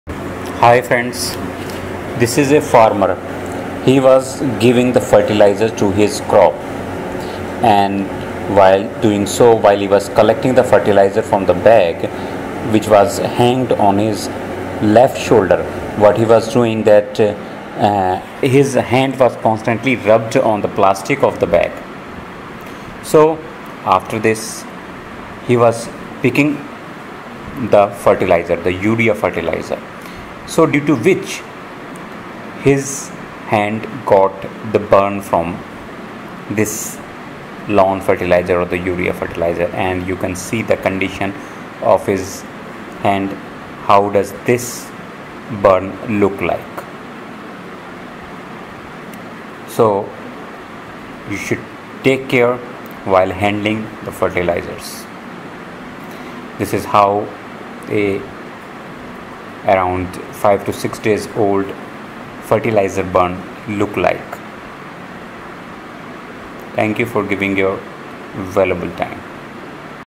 Fertilizer Burn On Skin
This video educates about the burn caused by fertilizer, Urea fertilizer was given to crop by this farmer few days back and he got such a burn as you can see in the video. So proper cleaning of wound is required with maintenance of hygiene so that it can be healed properly.